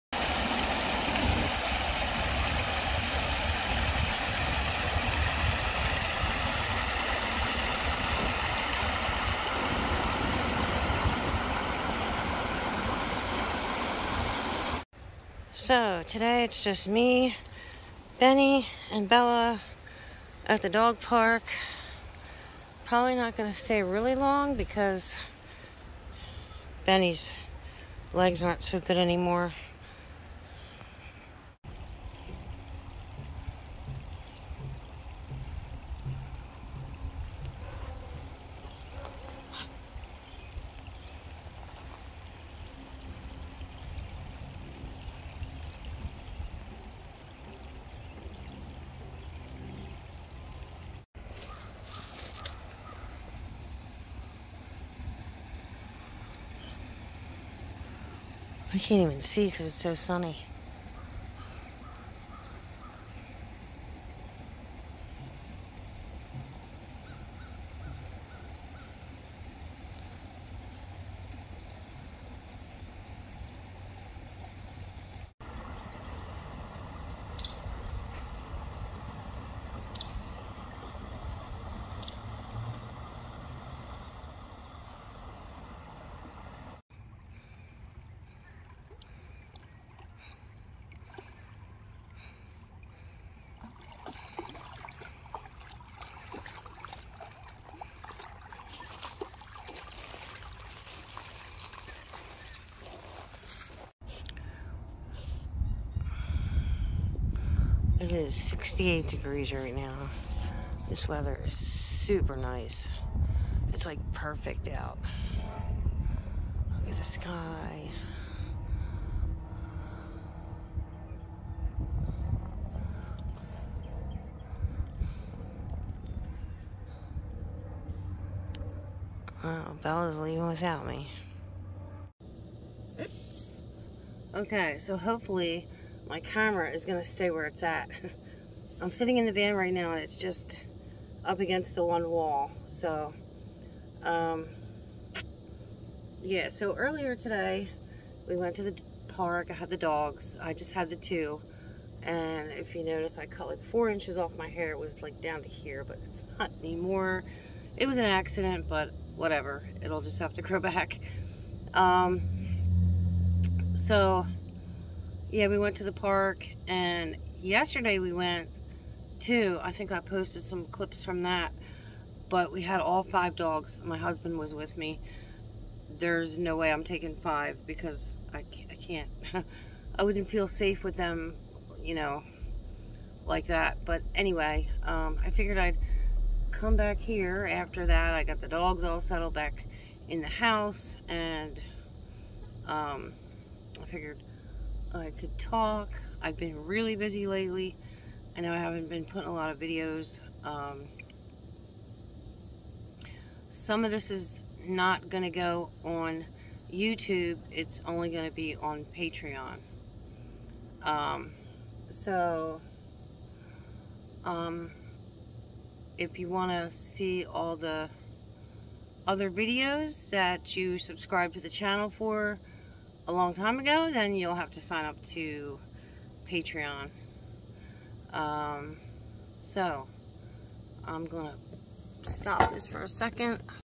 dog park and van chat.
At the dog park. Van chat at the end.